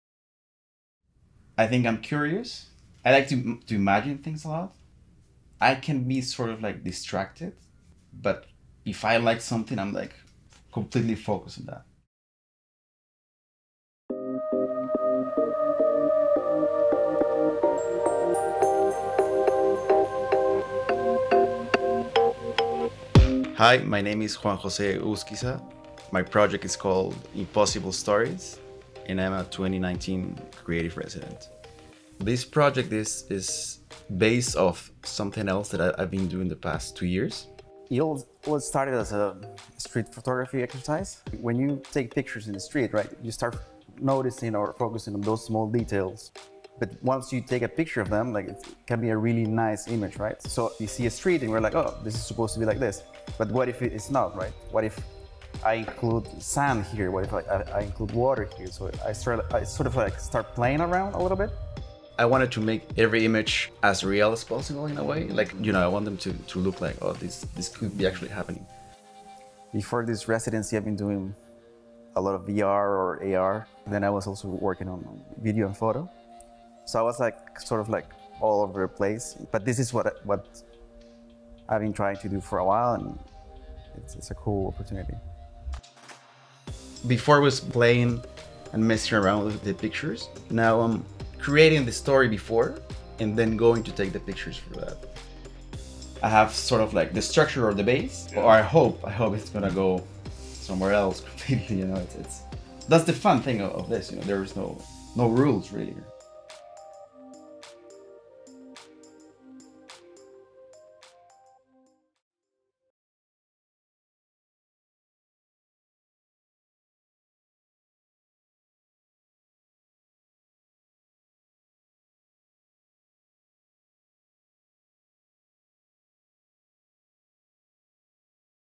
Meet the 2019 Adobe Creative Residents: Juan Jose Egusquiza | Adobe Creative Cloud
Juan Jose’s Impossible Stories project is a series of images from daily life that are manipulated to create a fictional reality. His residency is enabling him to share his creative process, get feedback from the community, and expand his storytelling tools and techniques. In a few years, he’d like to be a creative director whose focus is on making ideas visually tangible.